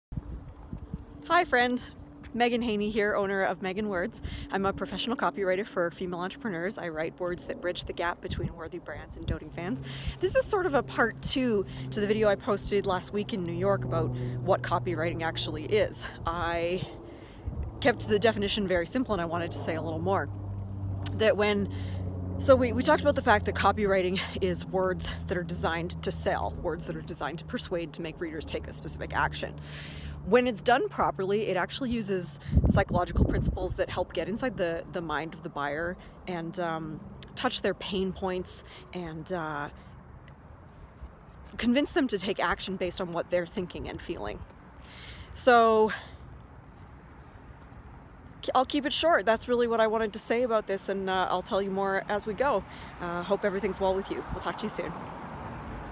How copywriting works
Wondering how copywriting actually works? What are the secrets? Quick answers here.

Does this video leave you feeling enthused about bringing a copywriter into your life?